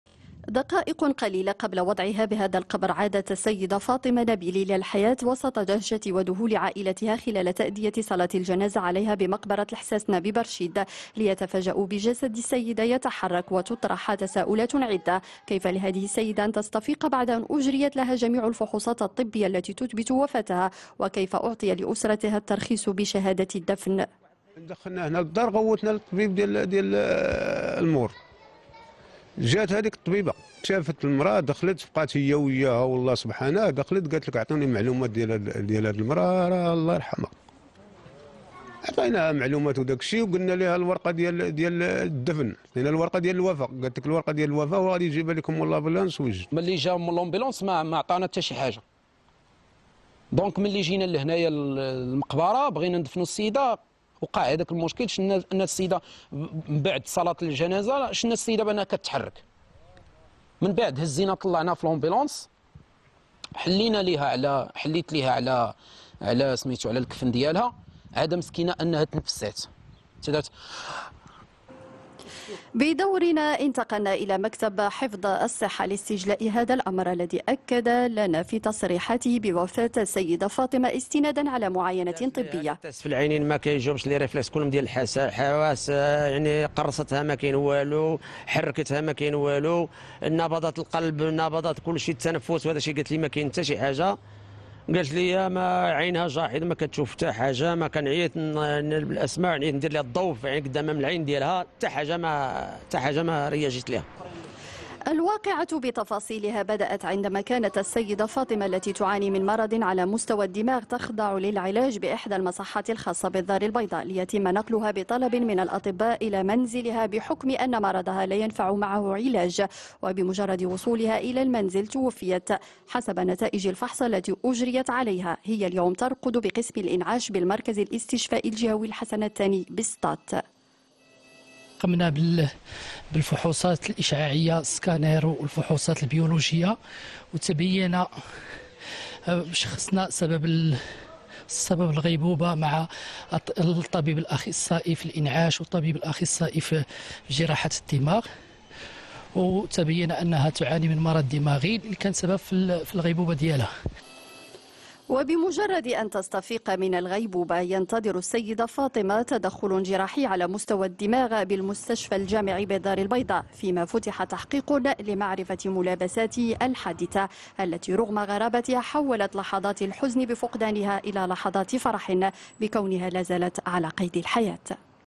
امرأة في عقدها السادس تعود الى الحياة وضعها في القبر
لا تزال امرة في عقدها السادس على قيد الحياة بالمستشفى الجهوي الحسن الثاني بسطات  بعدما جرى تغسيلها وتكفينها ومحاولة وضعها في القبر